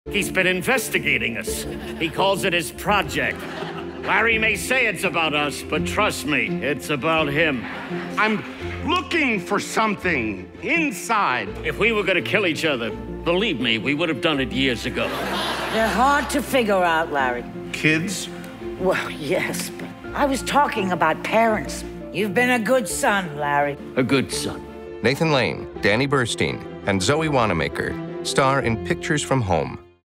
Pictures From Home on Broadway Through April 30th
Home is on Broadway at Studio54. Come see PicturesFromHome starring Nathan Lane, Danny Burstein, and Zoë Wanamaker.

Written by Sharr White (The Other Place) and directed by Tony Award® winner Bartlett Sher (To Kill a Mockingbird), Pictures from Home is a funny, poignant, and heartfelt portrait of a flawed but loving American family in a stage adaptation of groundbreaking photographer Larry Sultan’s captivating visual memoir.